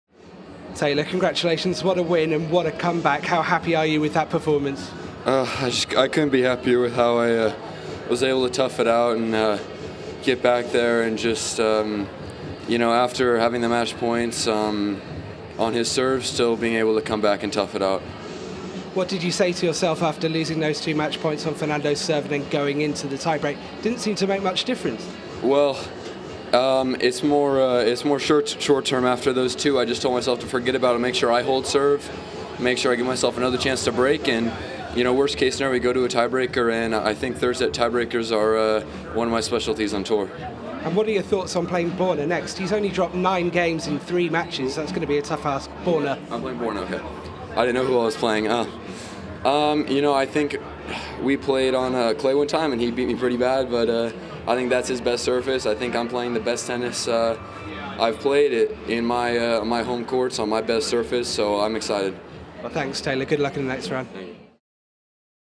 Fritz Elated To Get Past Verdasco In Indian Wells 3R 2018
Taylor Fritz breaks down how he snuck past Fernando Verdasco to reach the fourth round in Indian Wells. Watch live tennis at tennistv.com